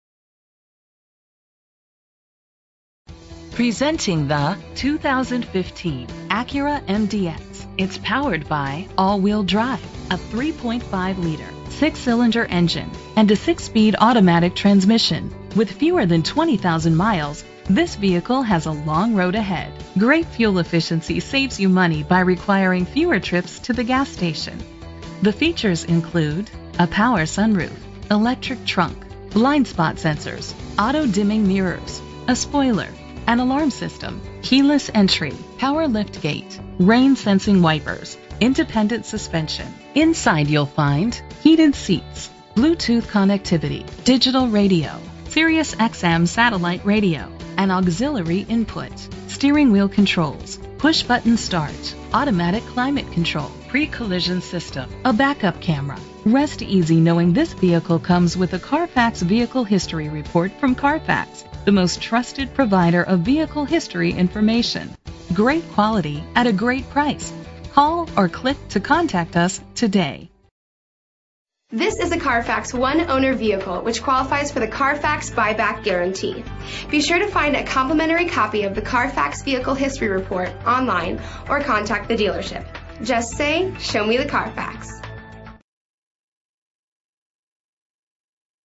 2015 Acura MDX P14043 - Hoffman Estates IL
Year: 2015
Make: Acura
Model: MDX
Trim: SH-AWD w/Tech
Engine: 3.5 liter 6 Cylindercylinder AWD
Transmission: 6-Speed Automatic
Color: Dk. Gray
Mileage: 19452
Address:
1099 W. Higgins Road
Hoffman Estates, IL 60195
VIN:5FRYD4H40FB004144